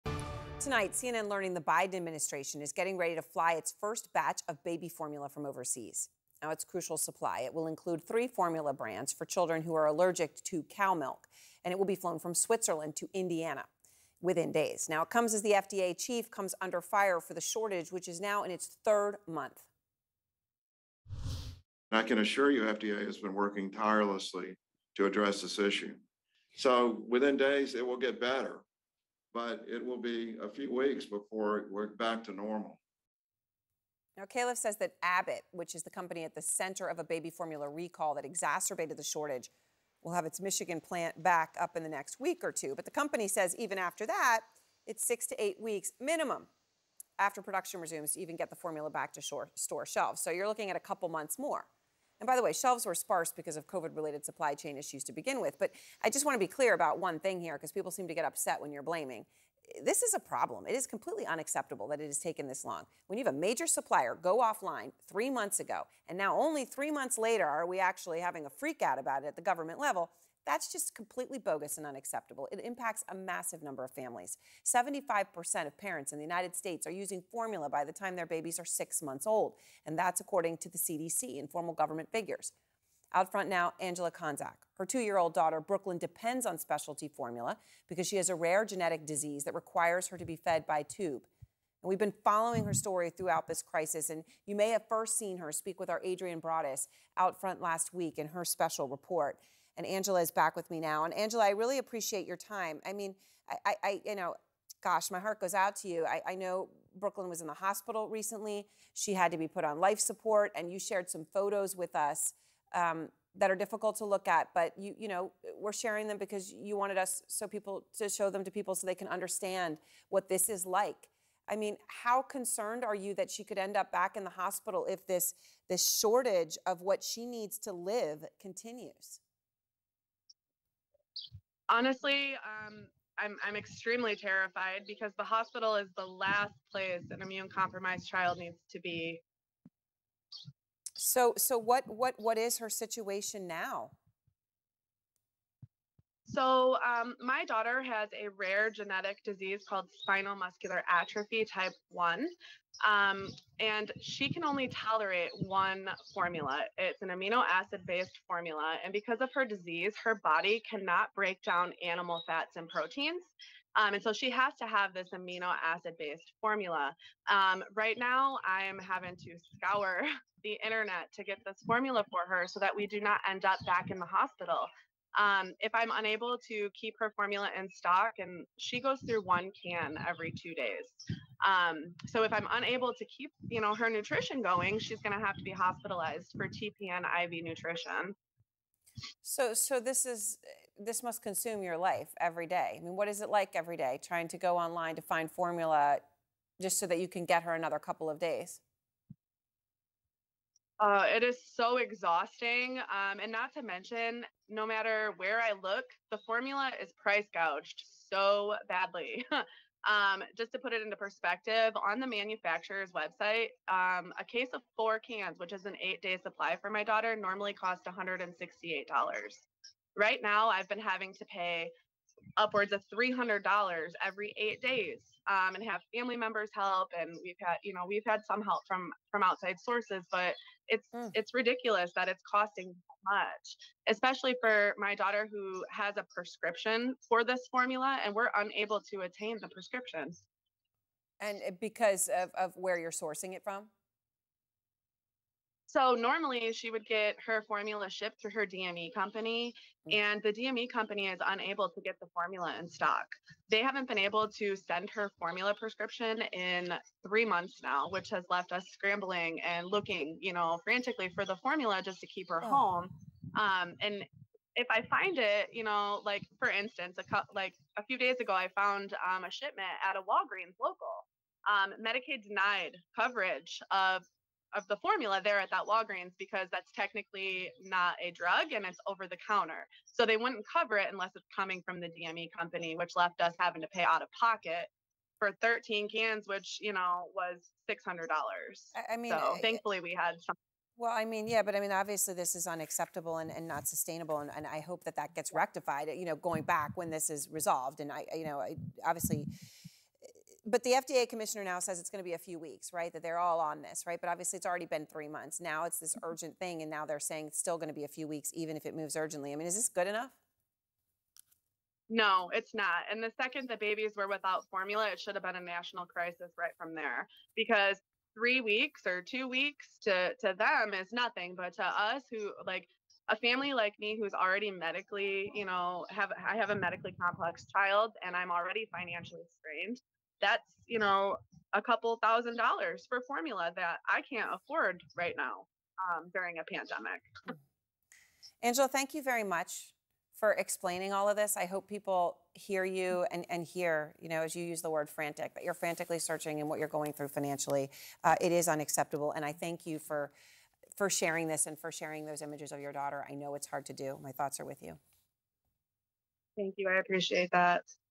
‘It is so exhausting’: Mother struggles to find special formula for her child
Angela Konczak’s two-year-old daughter, Brooklyn, depends on a special formula due to a rare genetic disease that requires her to be fed by a tube. Angela joins CNN’s Erin Burnett to discuss how the nation’s baby formula shortage makes it difficult for her daughter to stay out of the hospital.